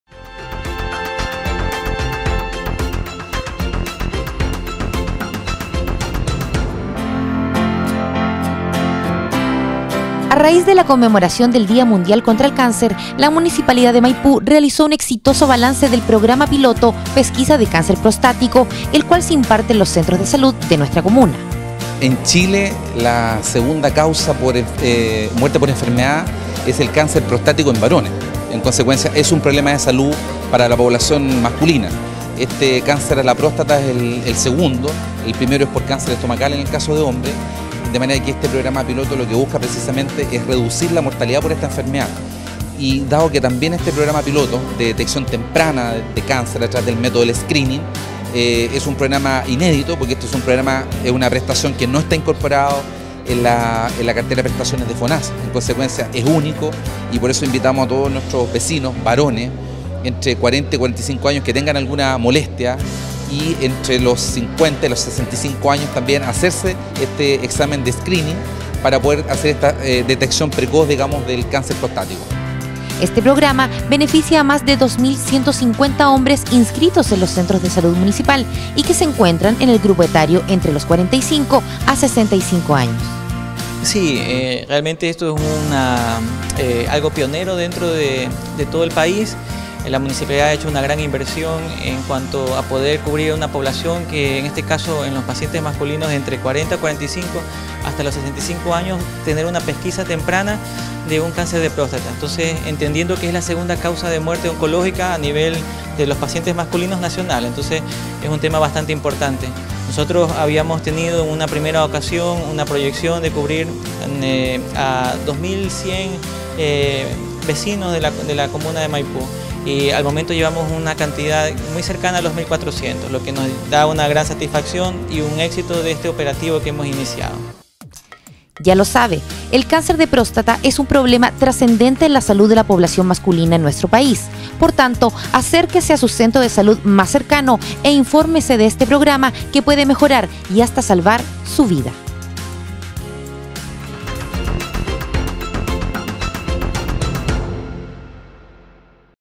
PREVENIR ANTES QUE CURAR
A raíz de la conmemoración del día mundial contra el cáncer, la municipalidad de Maipú realizó un exitoso balance del "Programa piloto Pesquiza de Cáncer Prostático", el cual se imparte en los centros de salud de nuestra comuna.